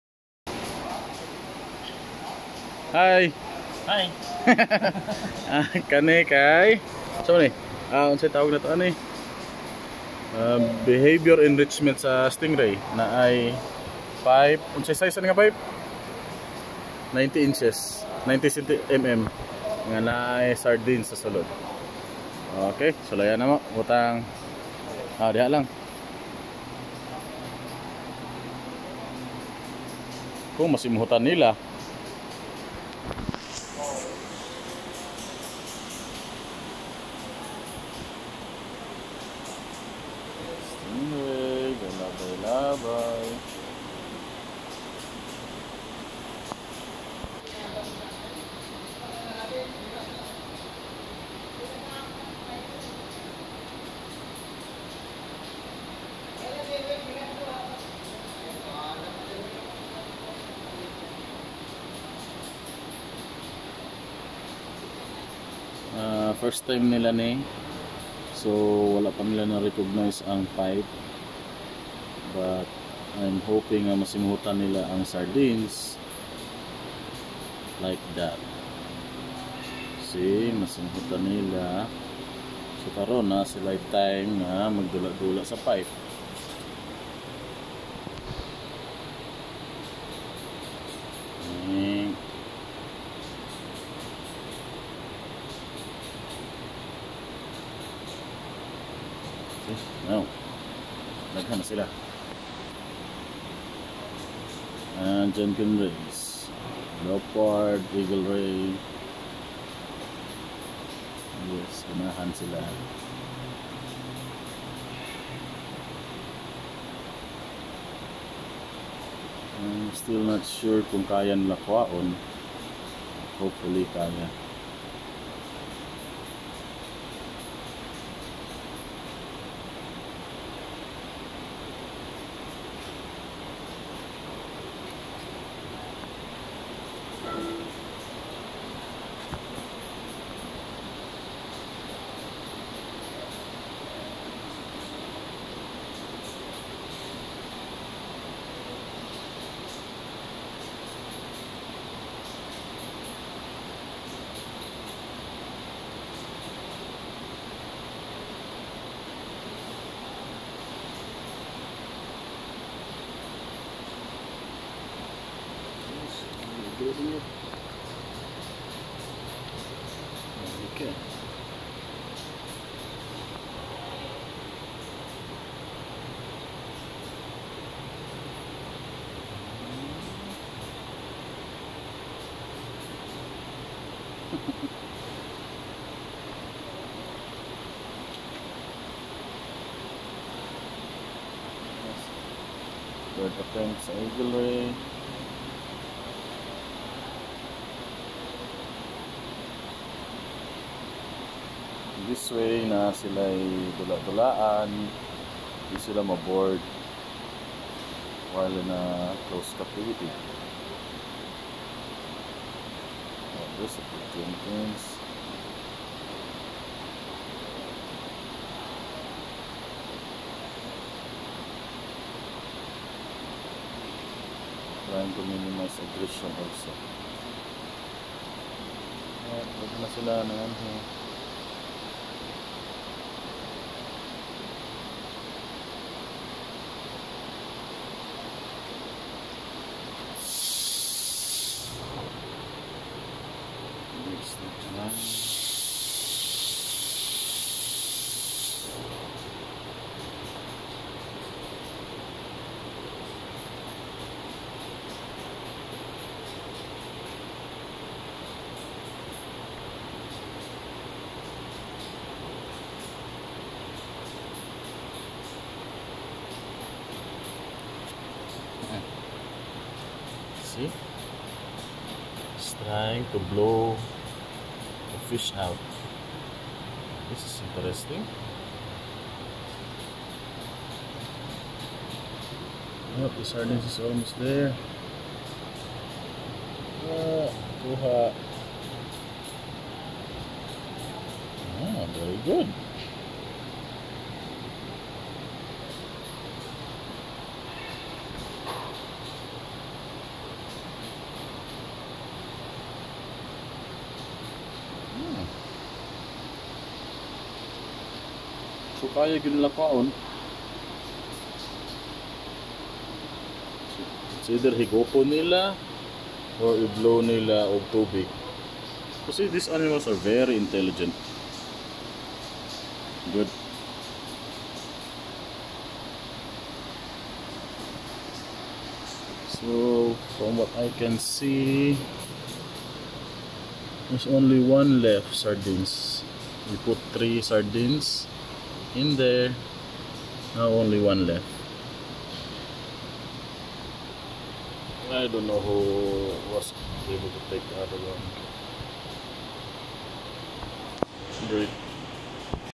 Stingray enrichment - Using a pvc pipe with sardines
Fish keeping / Aquarium / Fish husbandry